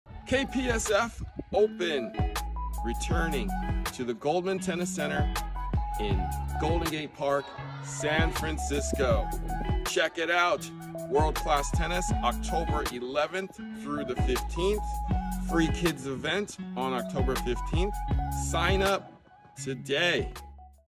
KPSF OPEN IS BACK AT THE GOLDEN TENNIS CENTER!
Get ready for the KPSF Open tennis tournament! Join us at the Golden Tennis Center in Golden Gate Park, San Francisco, from October 11th to 15th. Witness world-class tennis and don't miss the free kids event on October 15th.